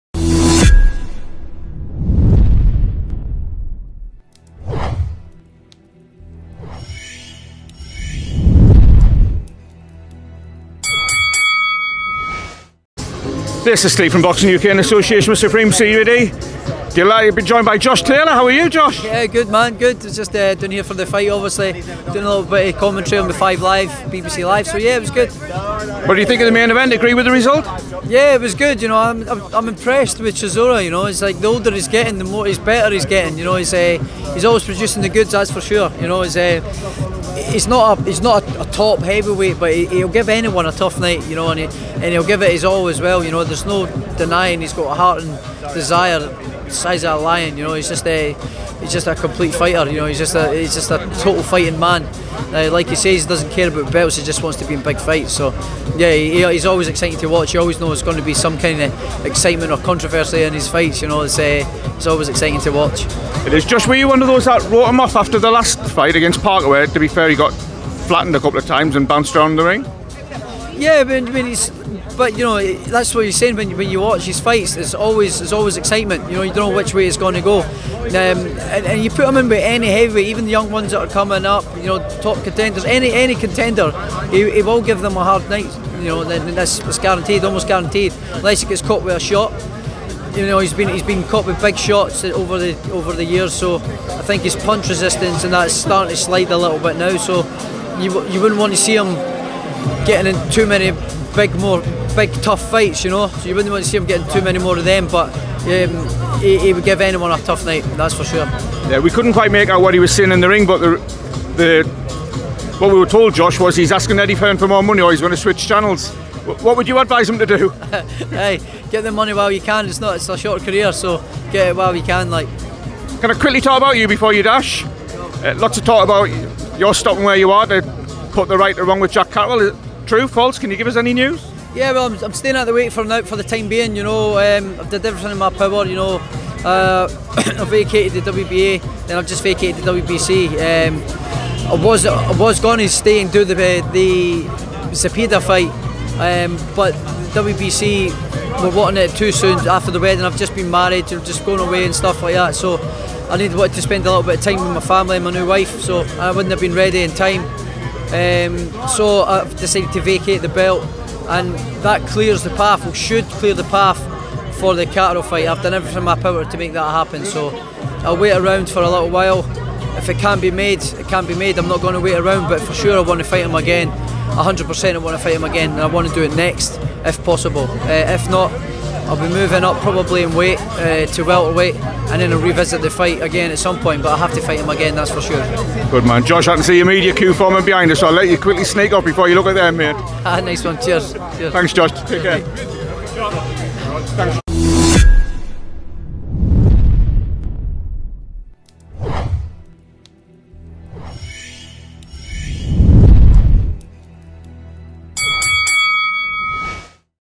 NO MORE TOUGH FIGHTS...PUNCH RESISTANCE SLIPPING!- JOSH TAYLOR REACTS TO CHISORA WIN/ CATTERALL 100%
Josh Taylor immediate reaction to Derek Chisora split decision win over Kubrat Pulev and confirms the Jack Catterall rematch is "100%" on!